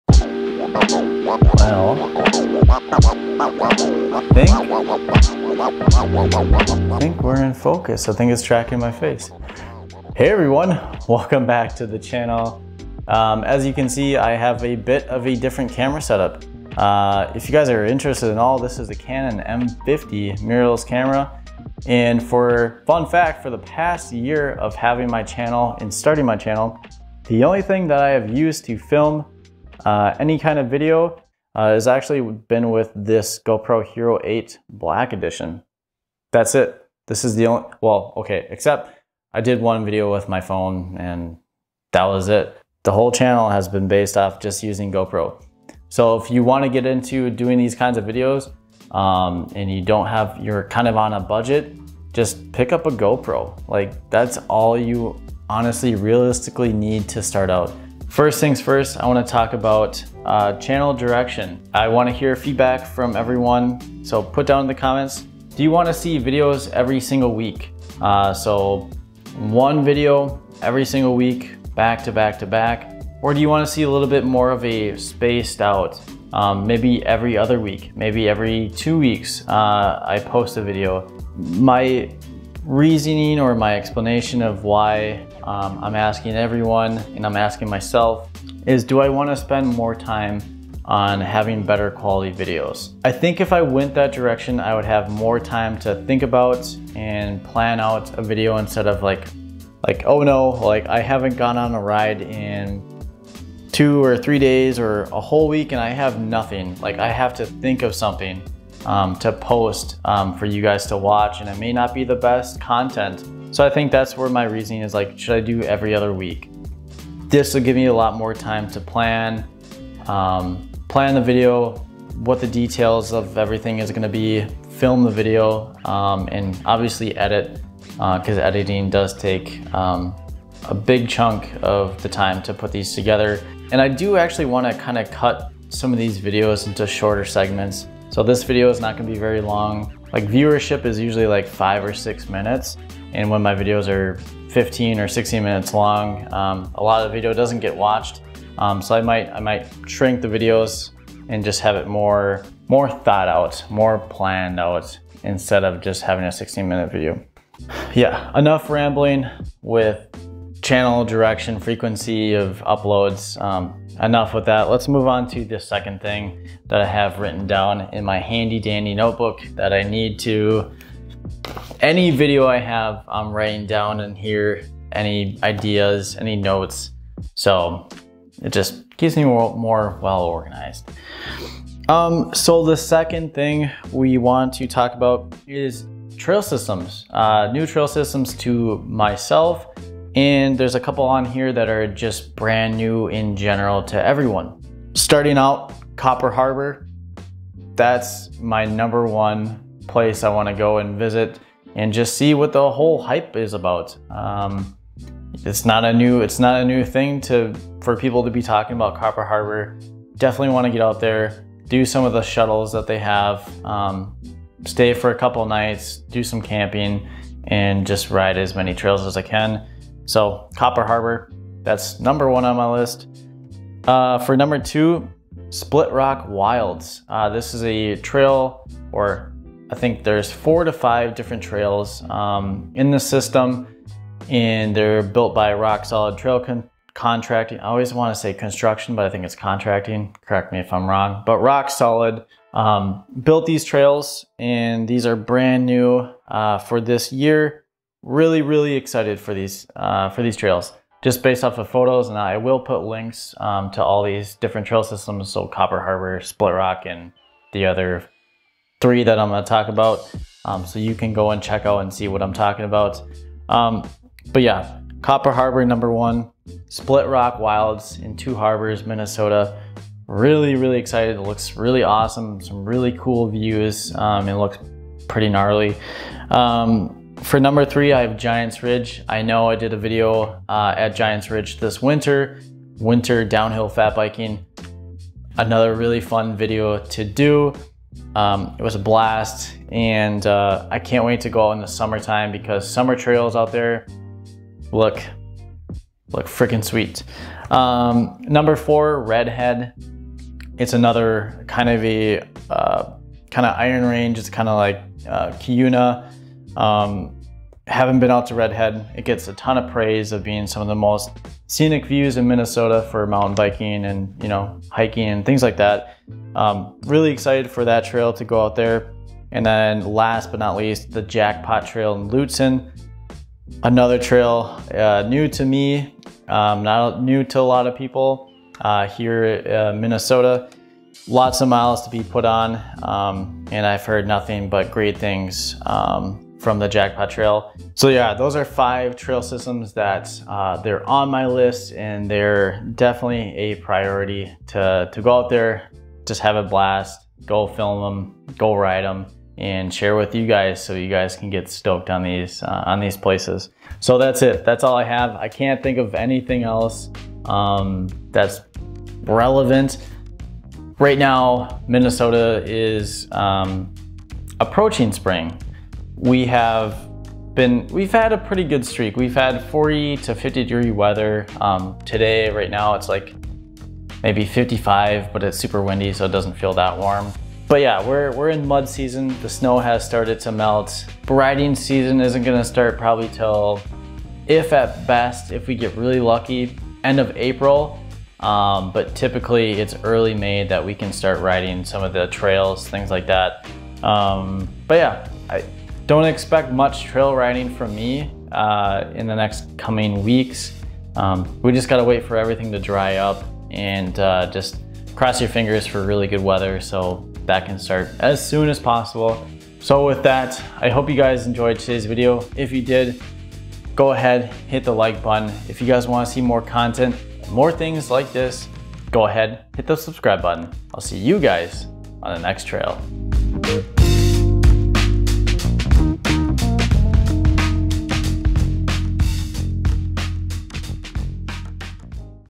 Getting excited for riding season! || Future plans & more!
Let's talk about what to expect for the channel this summer. The plan is to venture further out this year and ride some new trails. I can't wait for the trails to dry out and for the dirt season to begin!